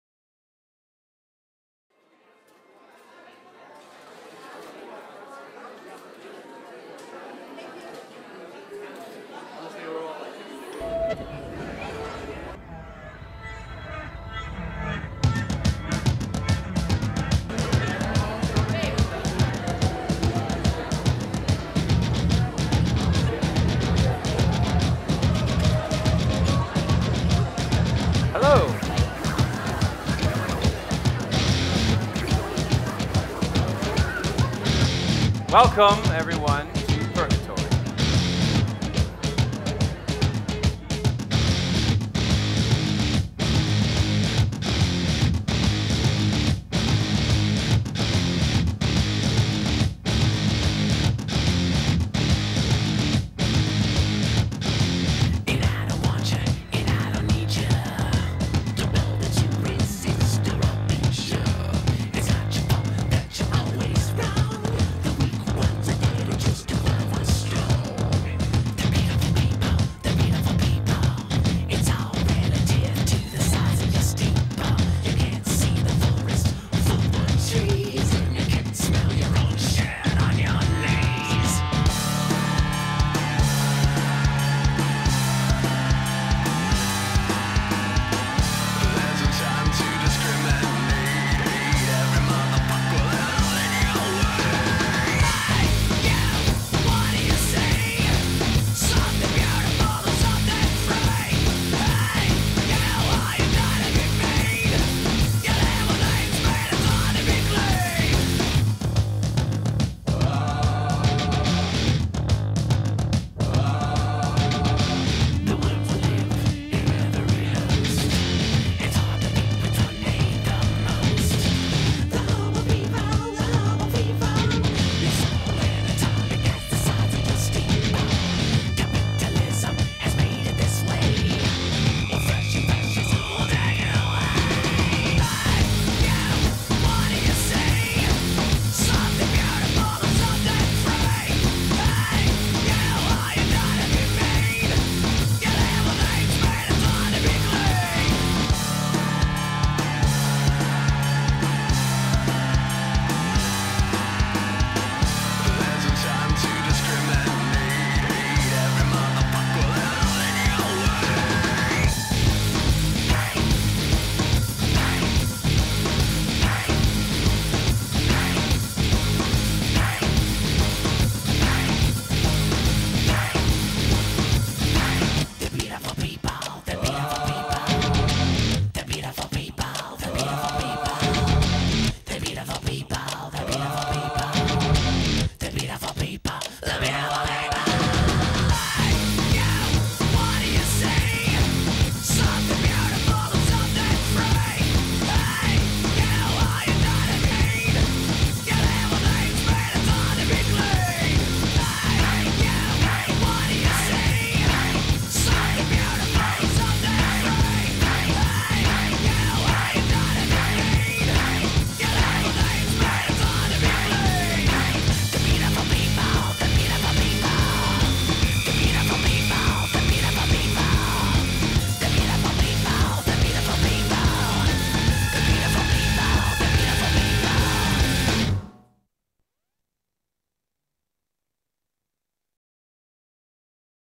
The Bone Orchard Follies
A taste of The Bone Orchard Follies presented by Les Coquettes on October 28th and October 31st, 2012.